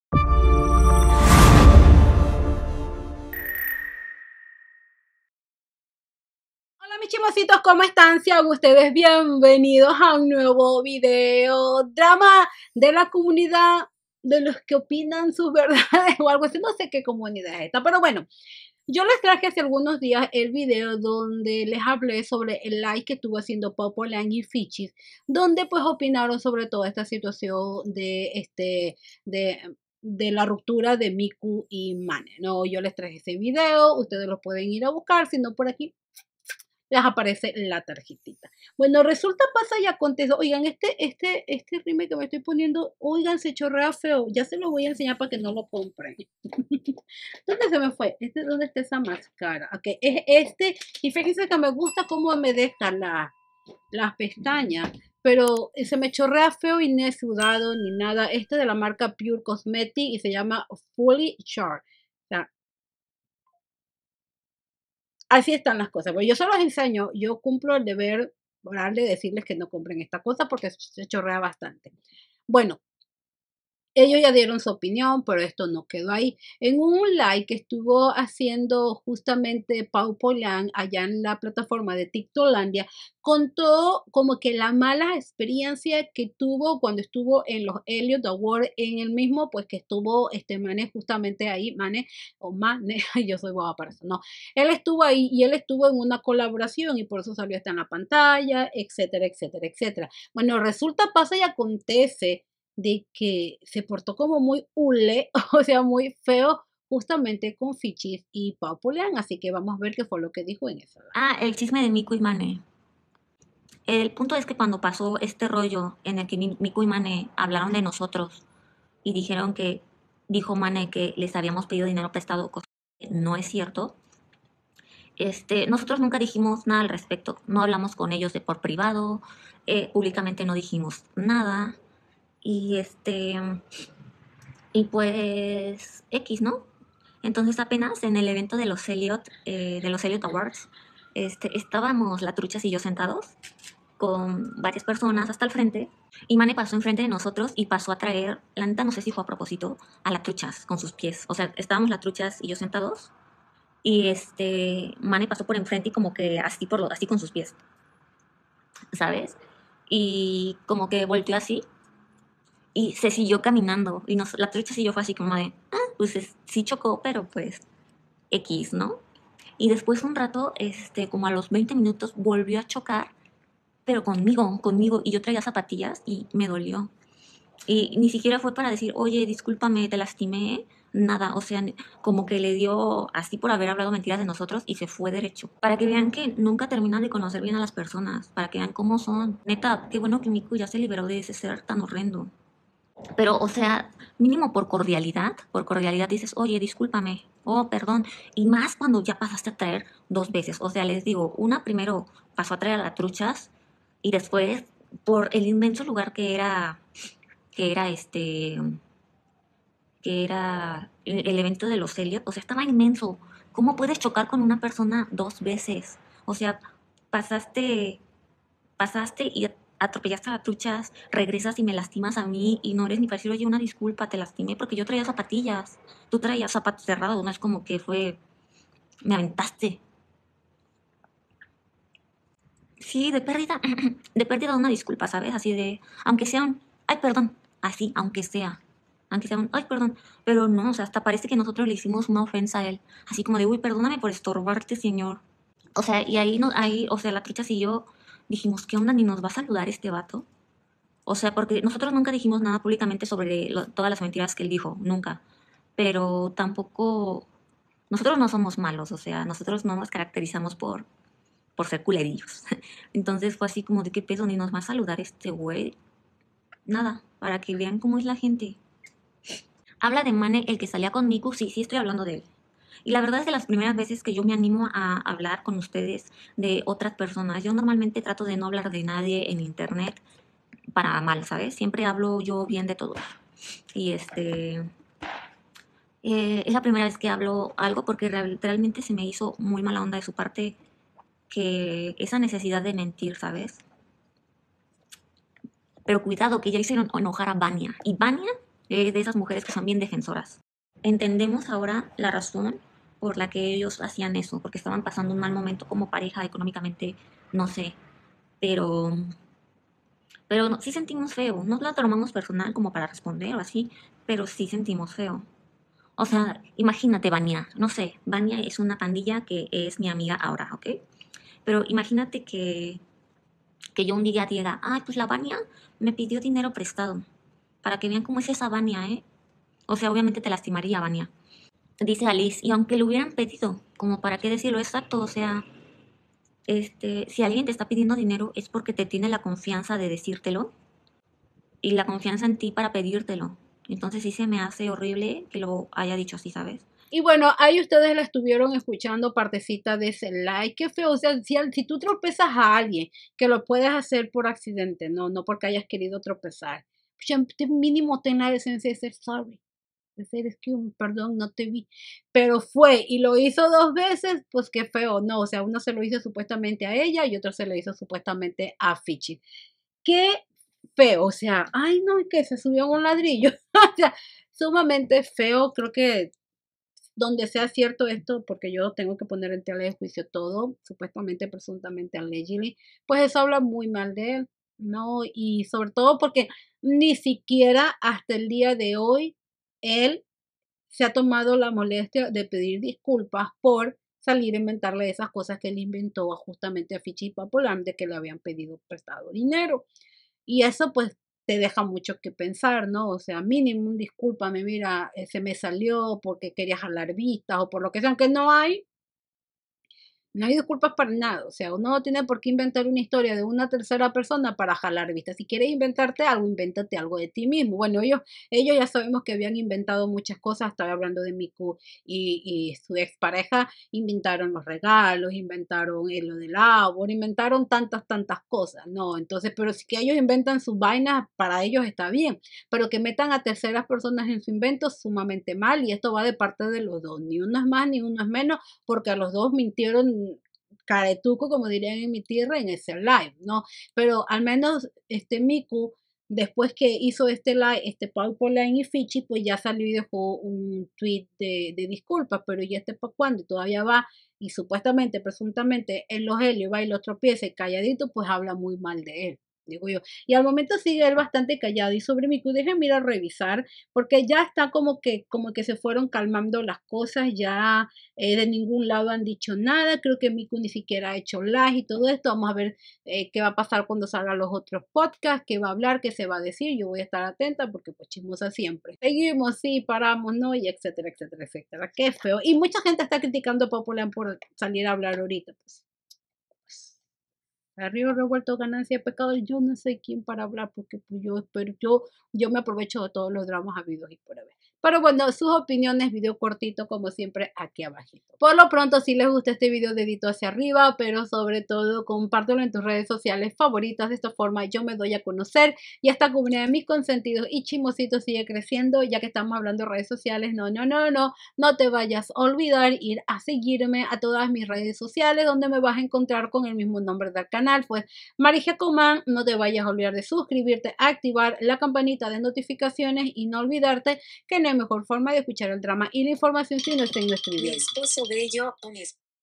PAO POULAINE CUENTA LA MALA EXPERIENCIA QUE TUVIERON CON MANE EN LOS ELIOTS AWARDS
𝙈𝙄 𝙀𝘿𝙄𝙏𝙊𝙍𝘼; 𝙆𝙖𝙧𝙚𝙣 𝙂𝙤𝙣𝙯𝙖𝙡𝙚𝙯
𝘗𝘢𝘳𝘢 𝘪𝘯𝘵𝘳𝘰𝘴, 𝘭𝘰𝘨𝘰𝘴, 𝘣𝘢𝘯𝘯𝘦𝘳𝘴, 𝘷í𝘥𝘦𝘰𝘴, 𝘦𝘵𝘤...